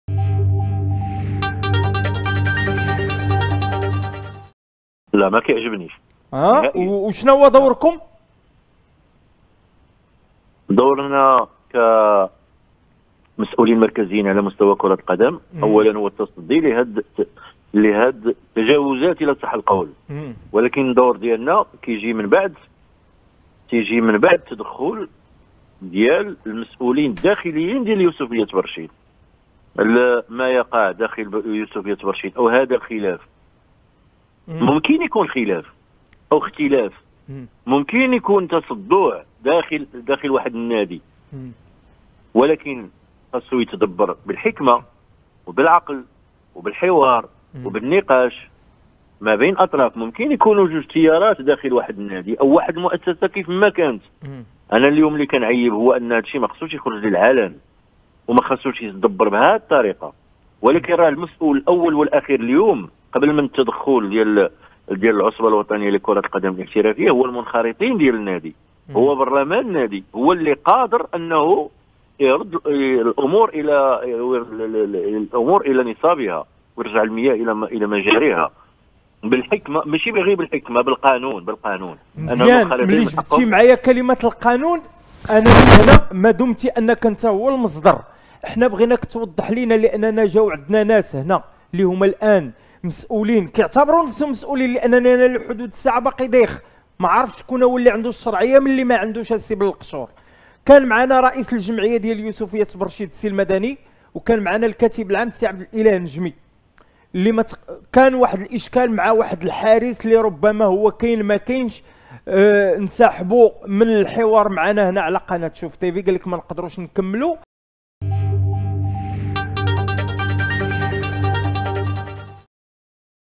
بلقشور: ما يقع حاليا في يوسفية برشيد لا يشرف كرة القدم الوطنية و تدخلنا كعصبة في حدود اختصاصنا
في الفيديو تصريح عبد السلام بلقشور رئيس العصبة الاحترافية حول ما وقع في يوسفية برشيد مؤخرا.